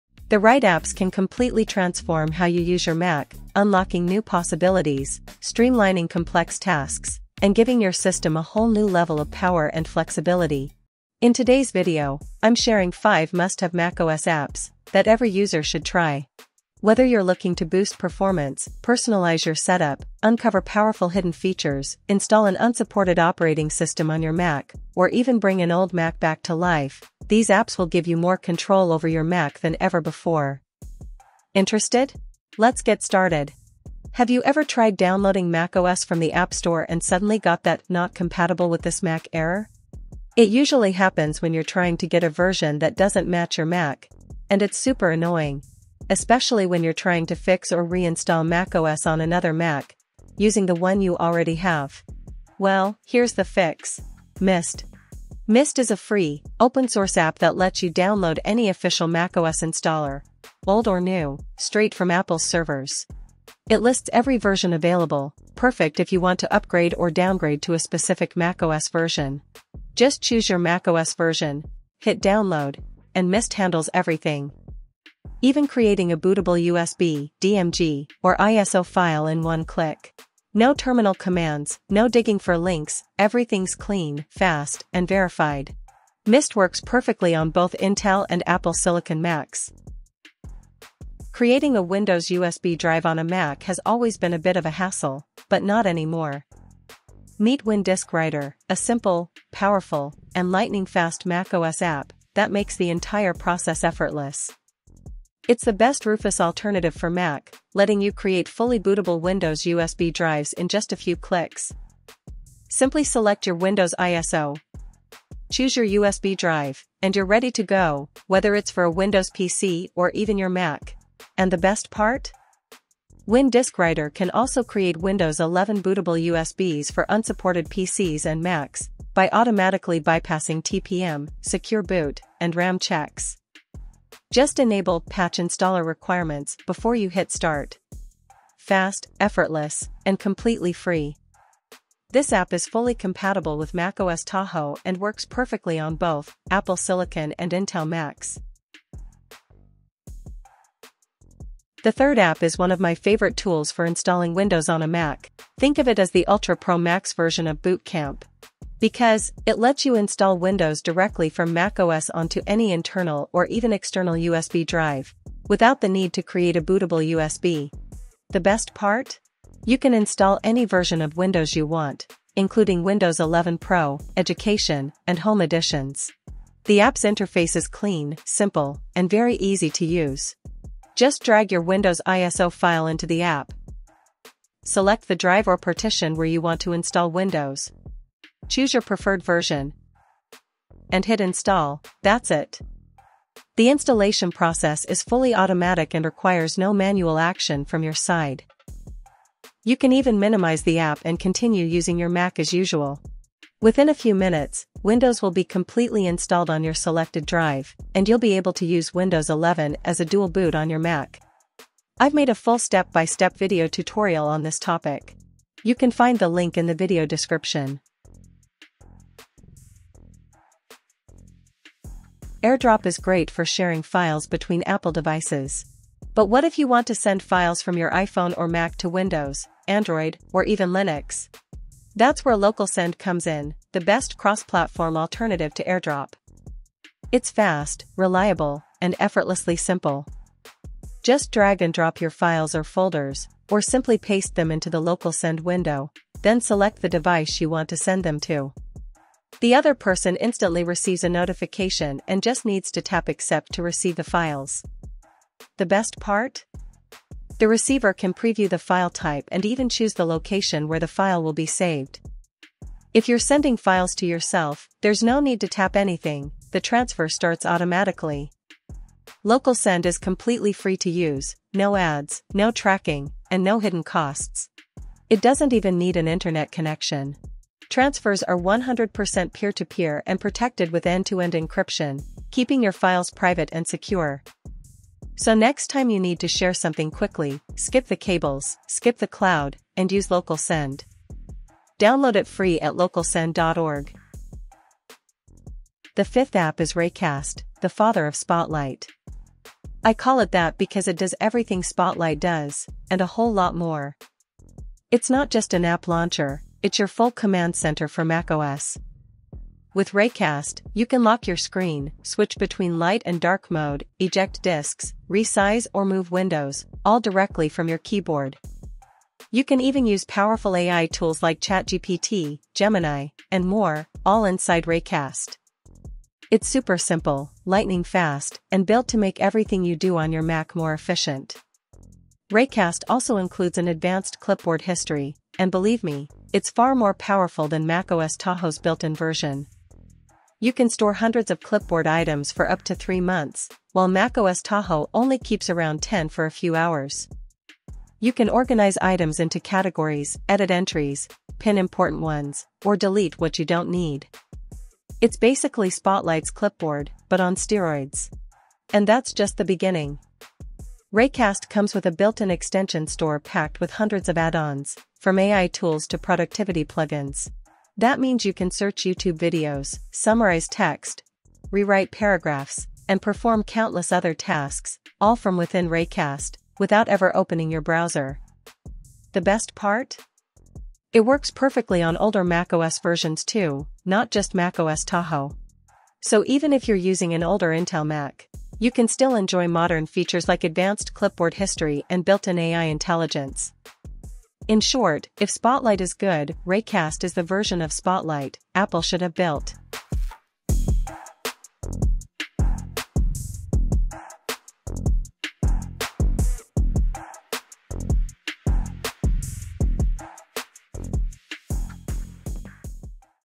Top 5 Free macOS Apps You Must Try in 2025 | Hidden Gems for Every Mac User
5 Best Free macOS Apps You Need to Try in 2025! 🚀

Looking for the most powerful and free Mac apps that can take your macOS experience to the next level? In this video, I’m sharing five must-have macOS apps that every Mac user should install right now, from downloading any macOS version, installing Windows without a USB, to sharing files across all devices instantly.

Apps Featured in This Video:

1:  Mist | Download any macOS installer (old or new) directly from Apple’s servers. Easily create bootable USBs, DMG, or ISO files. No Terminal, no hassle. Perfect for reinstalling or fixing macOS on any Intel or Apple Silicon Mac.

2:  WinDisk Writer (WDW) | The best Rufus alternative for Mac. Create Windows 10 or Windows 11 bootable USBs in just a few clicks and even bypass TPM, Secure Boot, and RAM checks automatically.

3:  Windows Install Tool | Install Windows directly on your Mac (internal or external drive) without making a bootable USB. Works perfectly with macOS Tahoe, Intel, and Apple Silicon.

4:  LocalSend | The best cross-platform AirDrop alternative. Share files wirelessly between Mac, Windows, Android, iPhone, and Linux. Completely private, offline, and free.

5:  Raycast | The ultimate Spotlight replacement. Launch apps, run commands, control your system, manage clipboard history, and even use AI tools like ChatGPT and Gemini, all from your keyboard.

Whether you’re using a MacBook Air, MacBook Pro, or iMac, these free macOS utilities will make your system faster, smarter, and more powerful. Perfect for students, creators, and professionals.

Each app in this list is free, lightweight, and safe, tested on macOS Tahoe and older versions too.